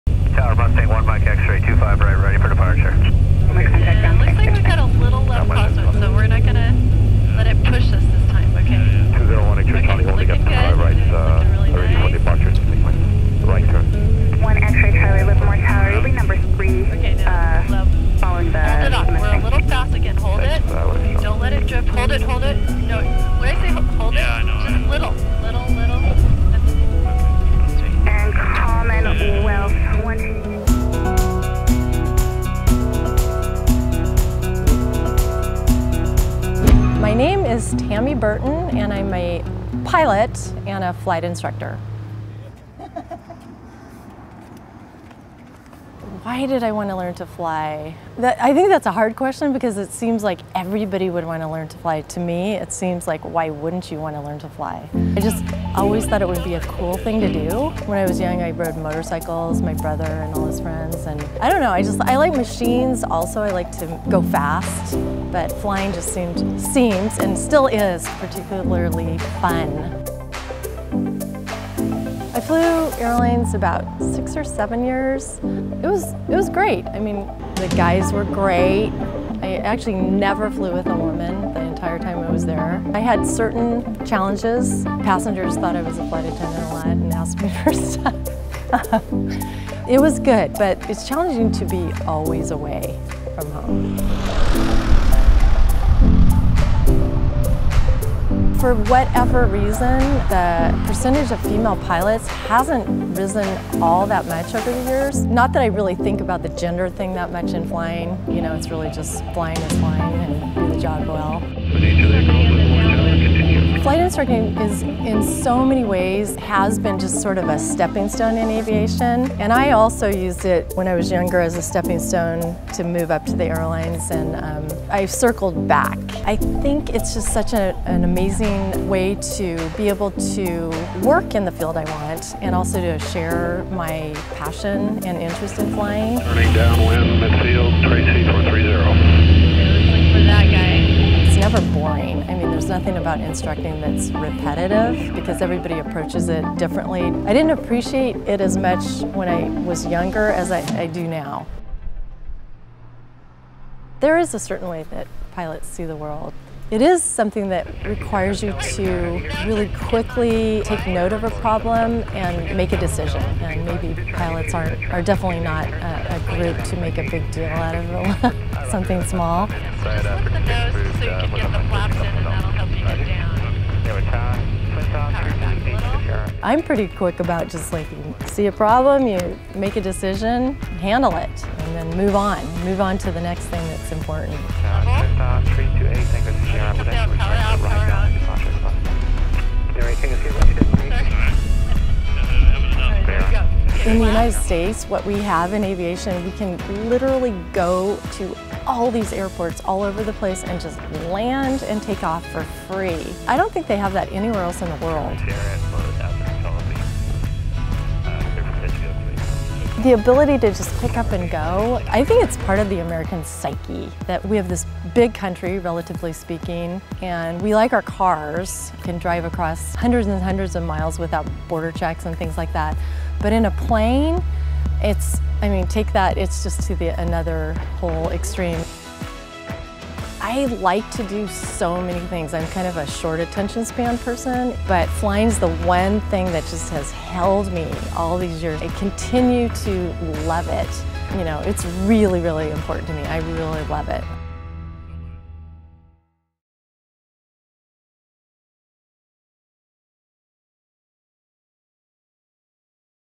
I’m a Pilot | My Life ★ Glam.com
Tammy Burton has always liked to go fast. Growing up, she fell in love with machines. She worked as a commercial pilot for many years and was frequently mistaken for a flight attendant. Today, she shares her passion for flying with others as a professional flight instructor. Learn more about a day in her life.